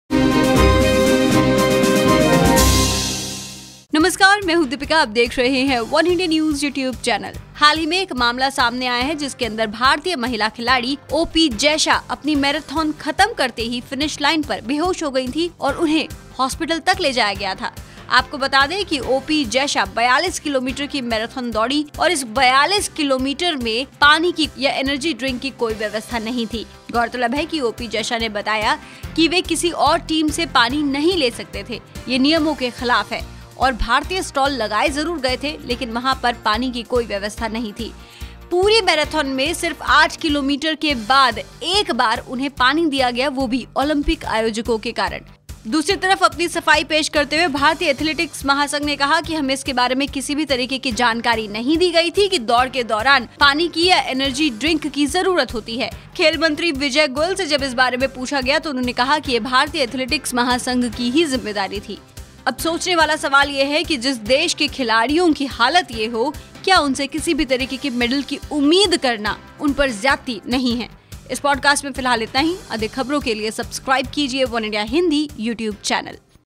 OP Jaisha fainted during marothon, AFI slams her for baseless allegations | वनइंडिया हिन्दी
Indian runner O.P Jaisha fainted after completing the marathon race at Rio Olympics 2016 on August 14. Apparently not a single Indian official who provided nothing, not even water to the Indian athlete. As per report Jaisha said: "Though there were officials from all other countries to provide refreshments to their runners at designated points after every 2.5 km, there was no one from India and our desks were empty next to the country's name and flag. The Indian runner had to run 42 Kms without a drop of water which caused weakness due to dehydration and she fainted.

रियो ओलंपिक में हिस्सा लेकर लौटी भारतीय एथलीट ओपी जाइशा ने बताया कि मैराथन के दौरान उन्हें तय जगह पर पानी नहीं मिला जिसकी वजह से उनके प्रदर्शन पर असर पड़ा और वह बेहोश भी हो गई।ओपी जाइशा ने बताया कि उन्होंने करीब 43 किमी. की दौड़ लगाई लेकिन इस दौरान उन्हें आठ किलोमीटर पर पानी मिल रहा था। जबकि दूसरे देशों के एथलीट्स को हर ढाई किलोमीटर पर पानी की व्यवस्था की गई थी।भारतीय एथलेटिक्स महासंघ ने कहा कि रियो में भारतीय अधिकारियों को एथलीट या उनके कोचों द्वारा किसी भी ड्रिंक की विशेष जरूरत के बारे में नहीं बताया गया था.